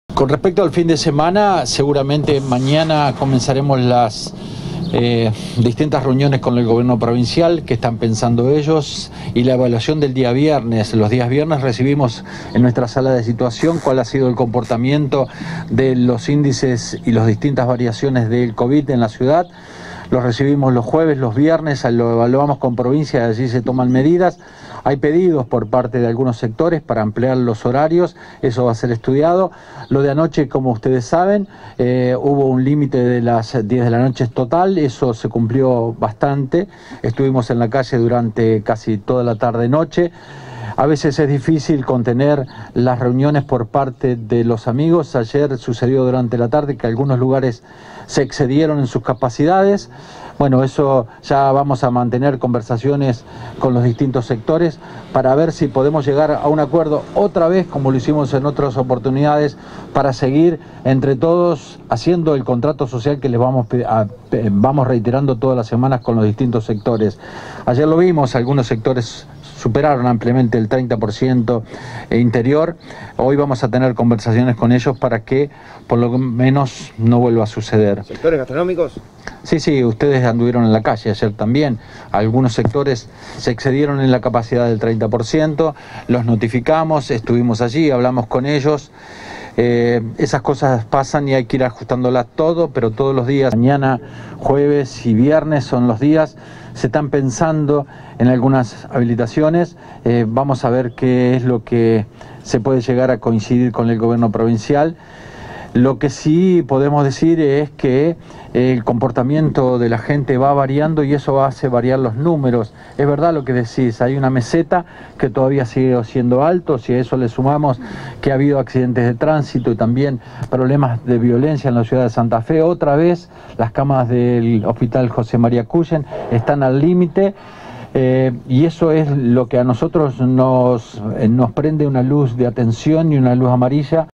LABRARON 10 ACTAS DE INFRACCIÓN DURANTE LA CELEBRACIÓN DEL DÍA DEL AMIGO
EMILIO JATÓN – INTENDENTE